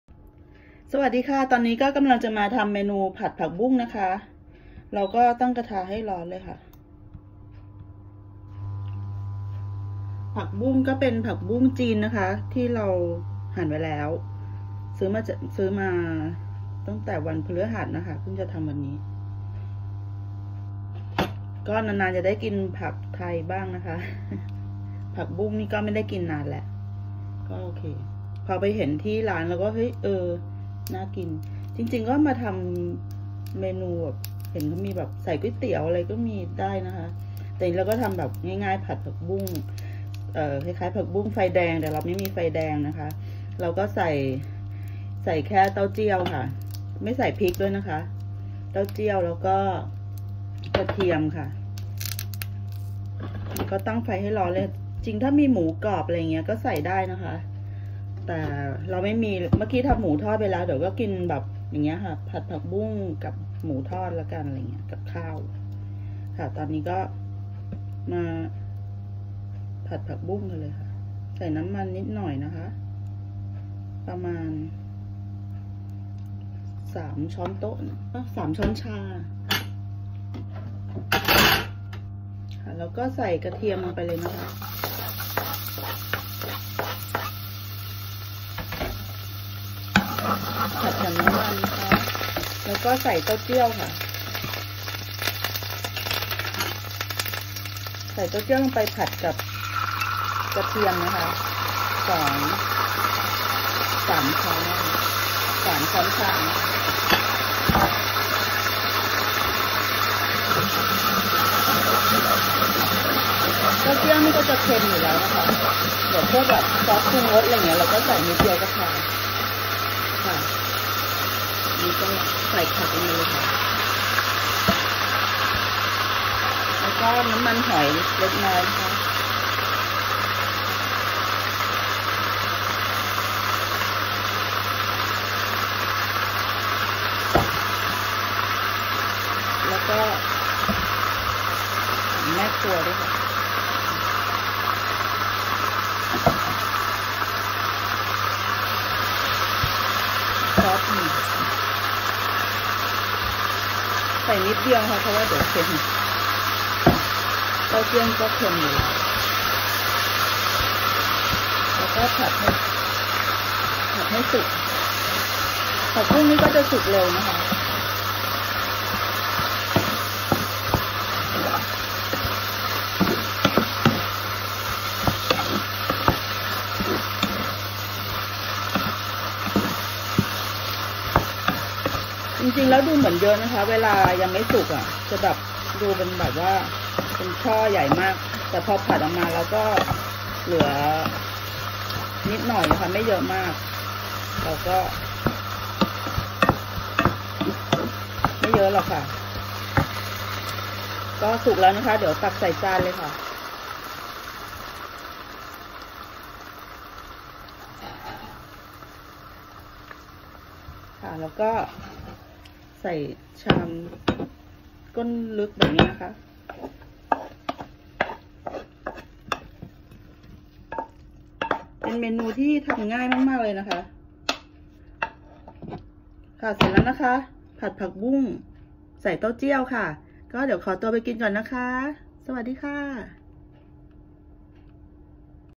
แม่บ้านไทยในสวิตเซอร์แลนด์ทำผัดผักบุ้งค่ะ
มื้อเที่ยงวันเสาร์ค่ะ ๑๖ มีนาคม ๒๕๖๒ ผัดผักบุ้งค่ะ